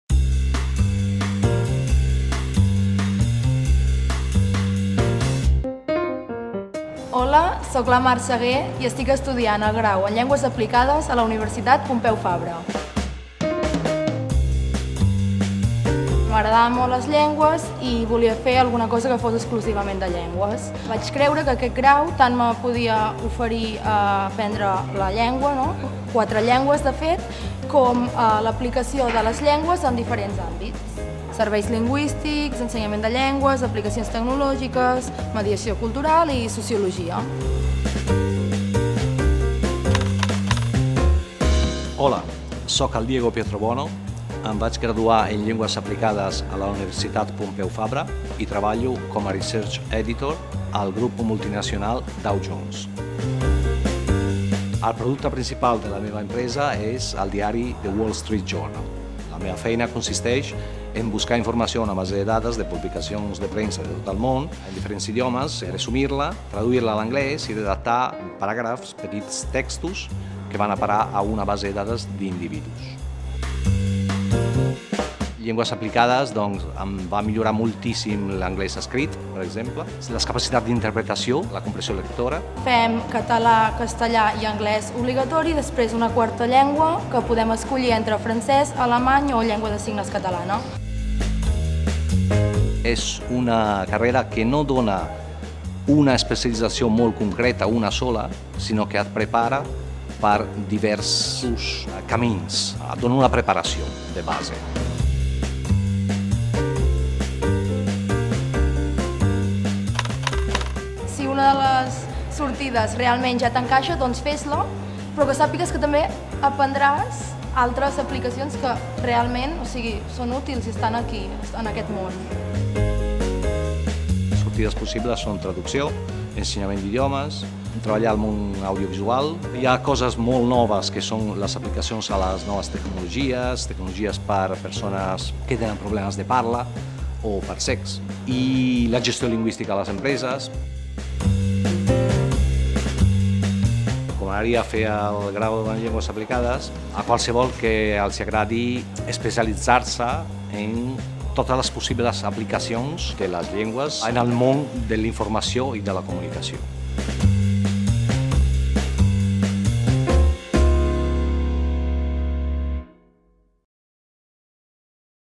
Grau en Llengües Aplicades
El grau en Llengües Aplicades forma professionals qualificats per gestionar la comunicació multilingüe i especialitzada en l'àmbit laboral. Garanteix el domini de les llengües pròpies i de dos idiomes, tant en les activitats verbals més rellevants (redacció, correcció, edició, traducció, expressió oral, mediació cultural) com en la utilització de recursos (gestió de la terminologia i de la documentació, informàtica aplicada, mètodes de recollida i de tractament de dades), i la reflexió sobre el llenguatge.